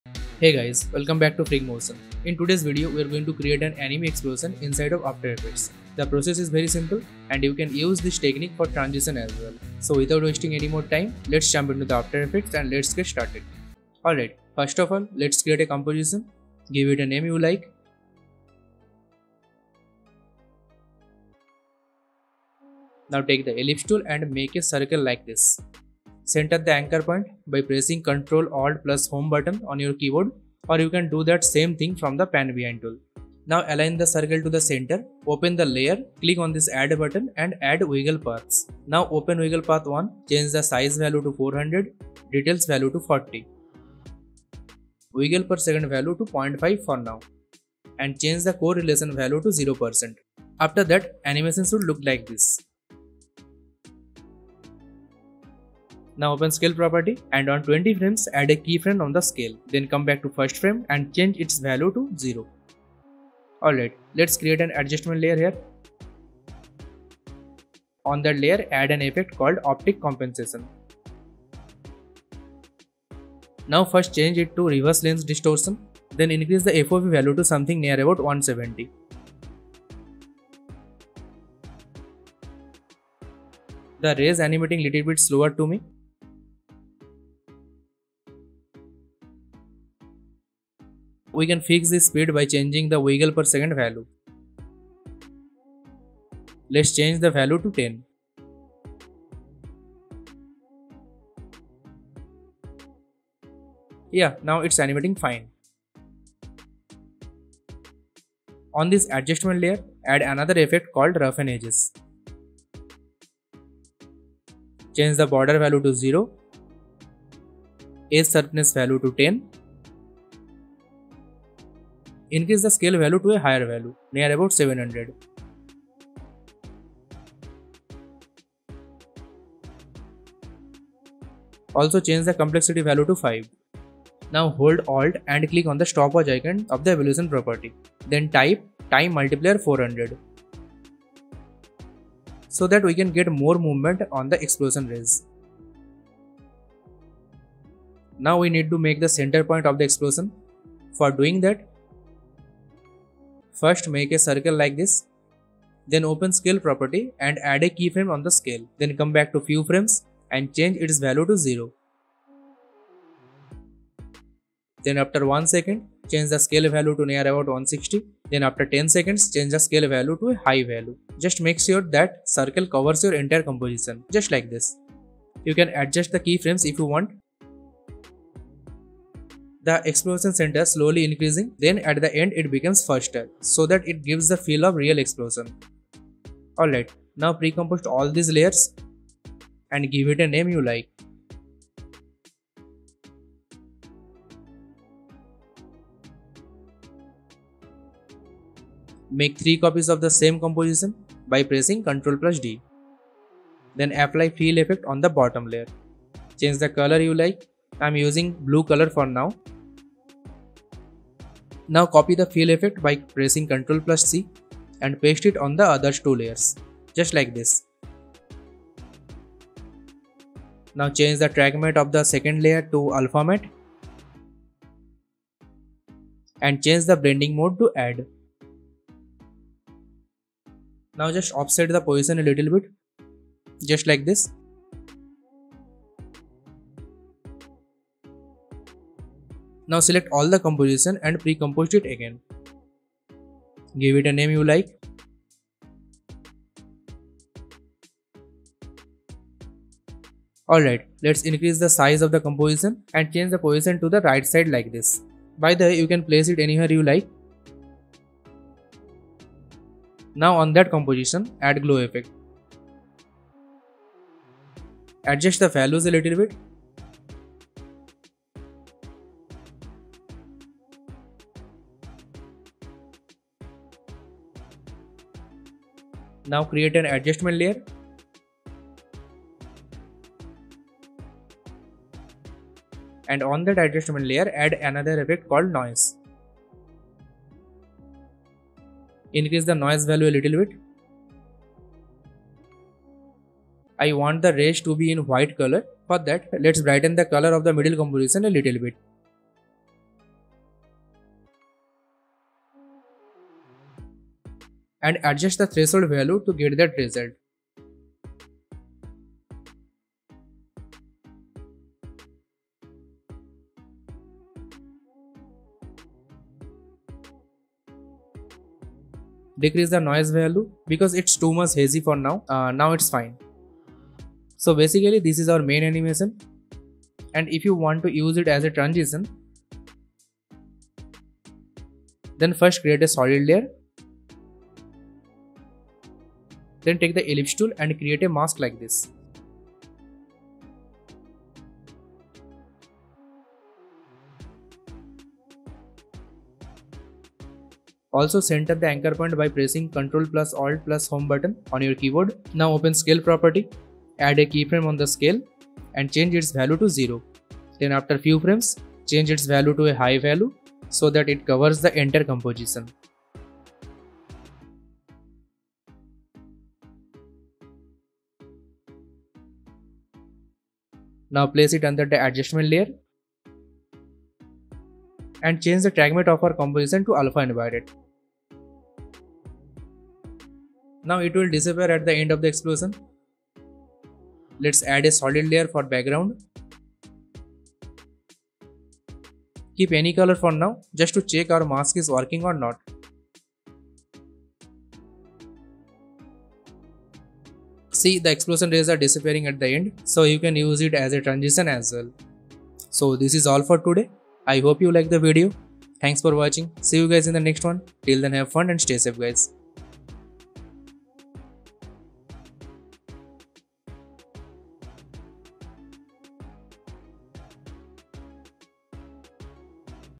Create Stunning Anime Explosions in After Effects: Tutorial and Tips - Step-by-Step Tutorial
Hey, everyone in this after-effects tutorial we will create Stunning Anime Explosions in After Effects. We will also cover topics like how to use this technique for transition as wel, and more!

🙌  A cup of coffee can make my day!